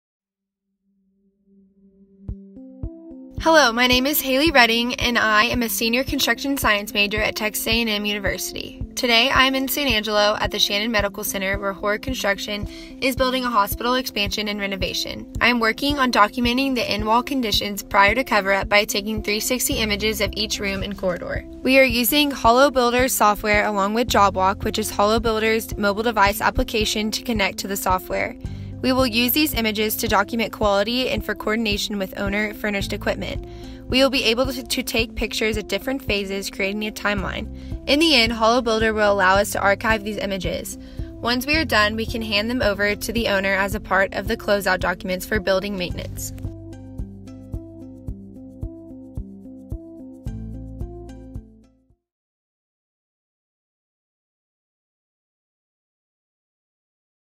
HoloBuilder at Shannon Medical Center
Texas A&M College of Architecture's Hayley Redding is an intern with us and has visited our job site at Shannon Medical Center to help document as-built conditions for the new tower we are building. Check out the HoloBuilder technology she's learning to use and how it will be helpful for the project moving forward!